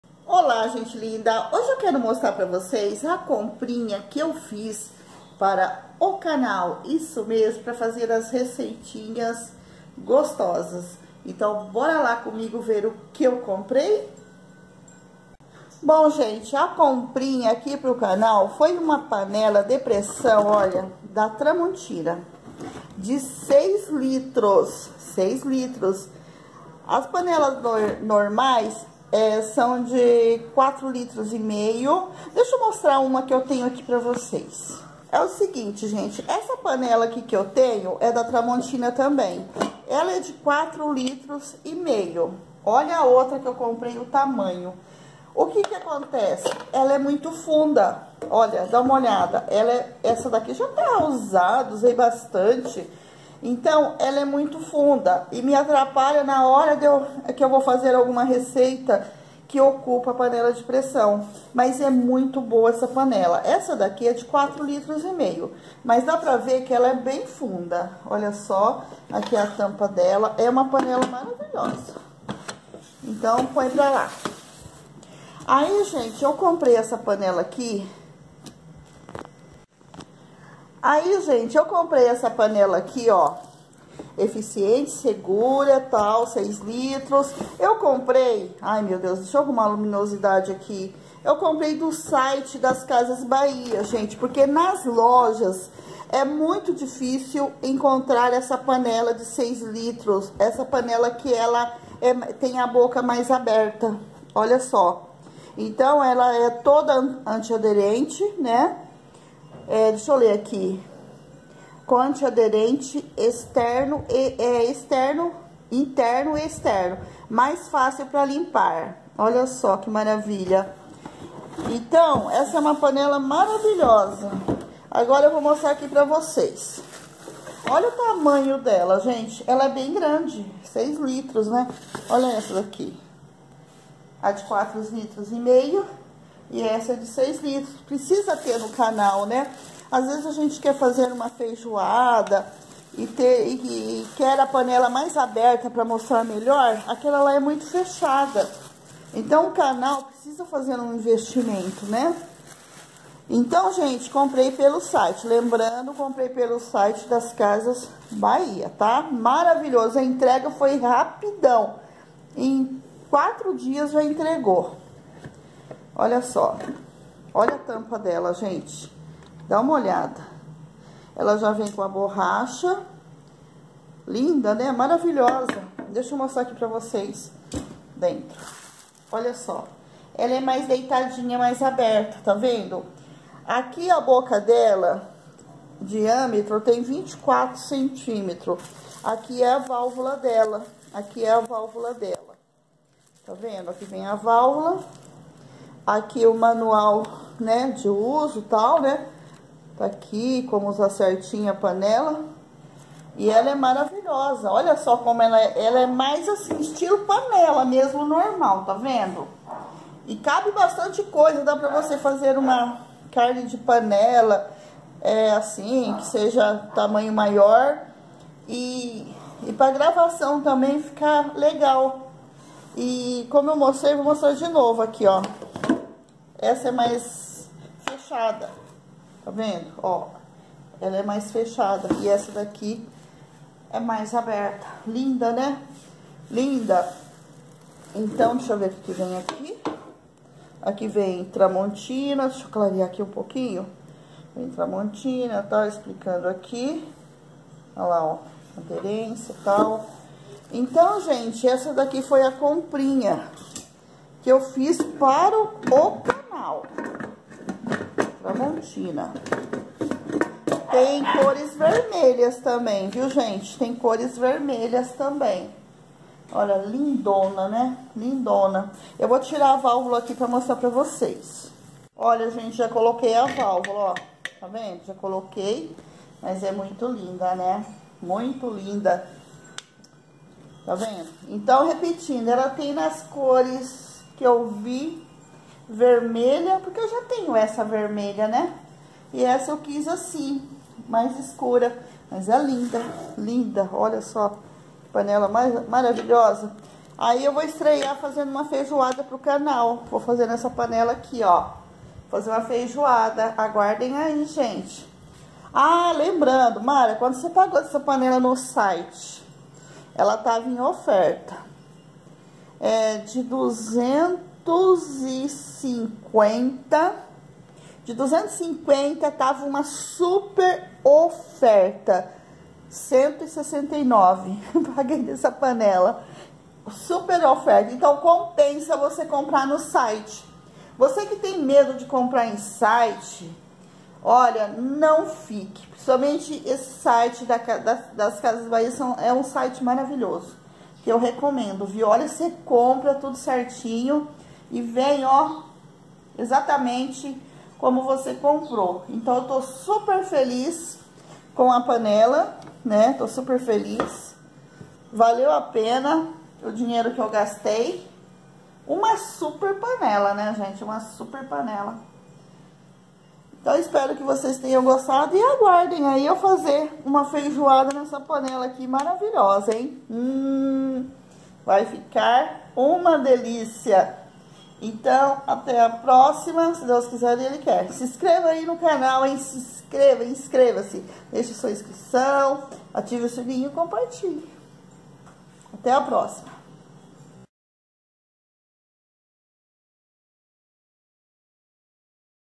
COMPRAS PARA O CANAL PANELA DE PRESSÃO TRAMONTINA 6 LITROS QUE ACHEI DO SITE CASAS BAHIA
OLA GENTE LINDA PANELA MARAVILHOSA SUPER EFICIENTE CONFIRA NO VIDEO.